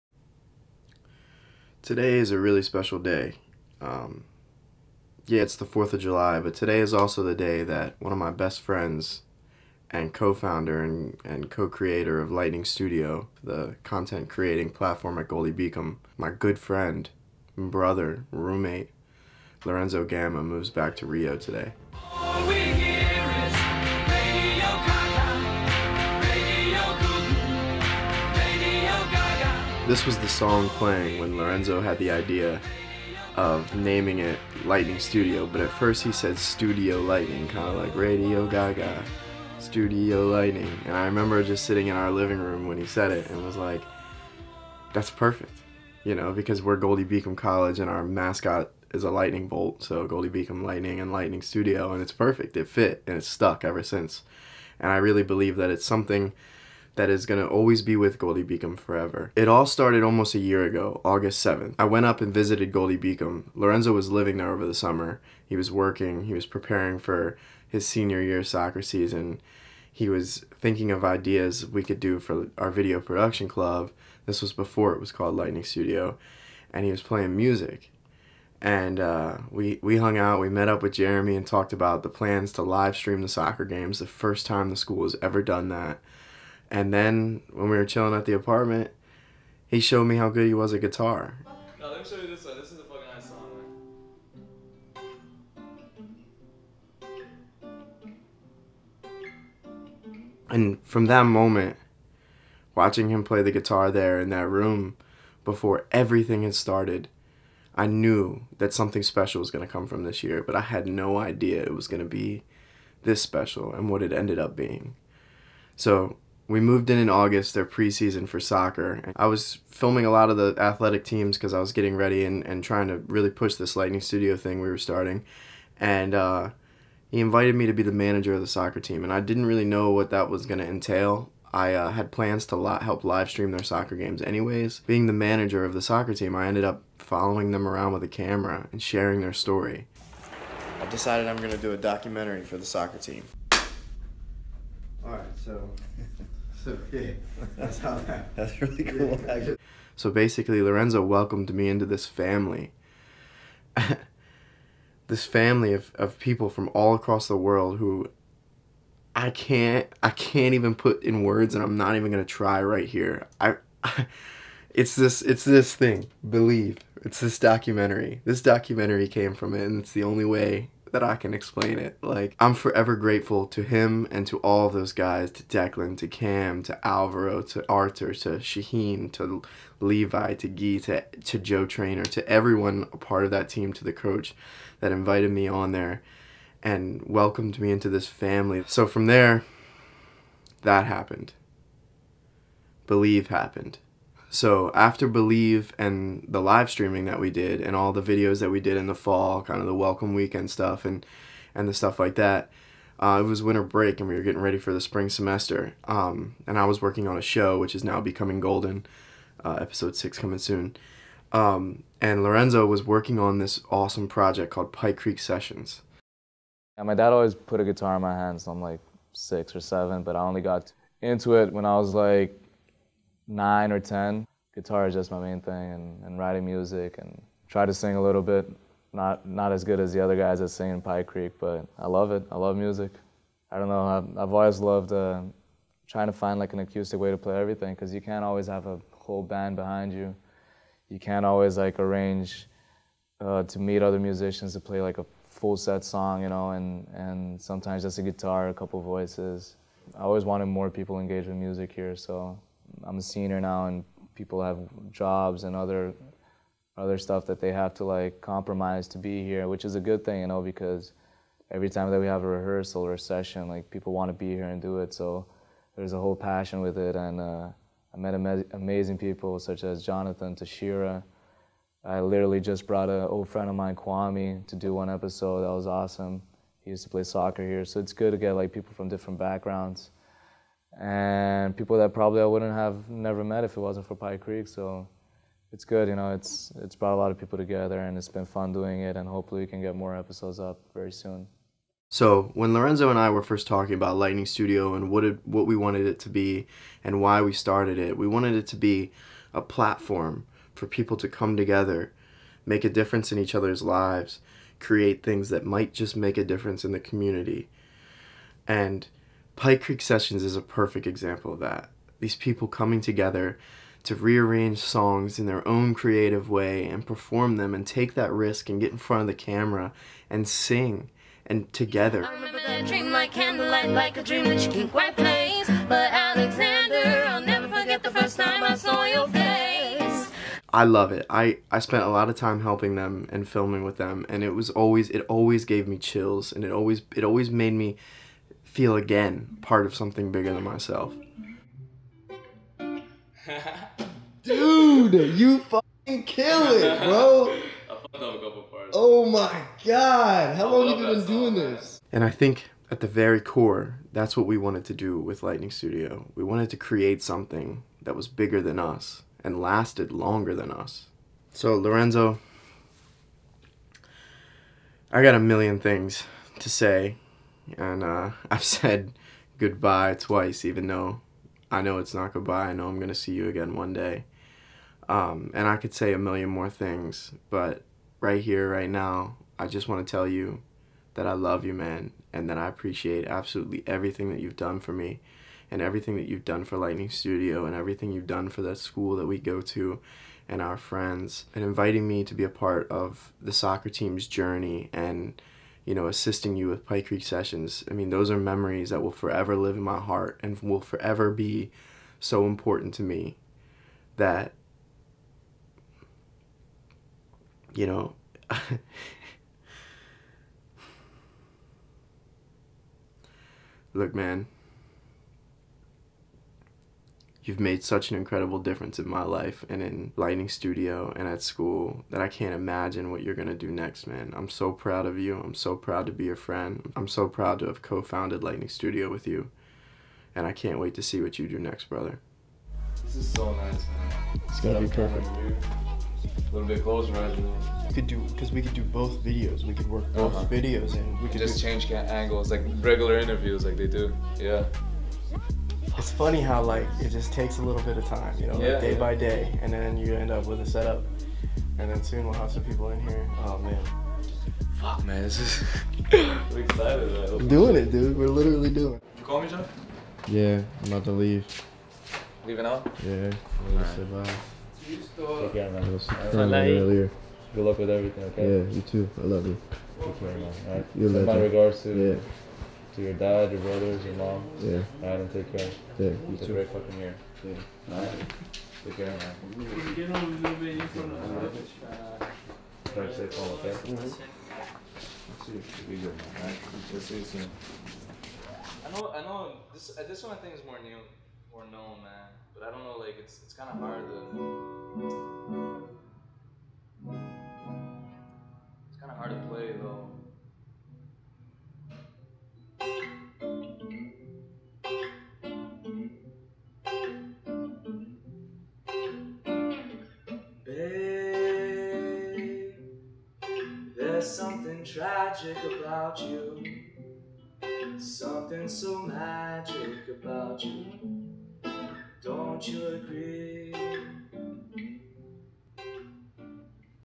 To my friend Lorenzo Gama (we will go again)
To a friend whose changed my life and whose legacy at Goldey-Beacom College will live on forever. Enjoy home, my friend. Until next time! Studio Lightning!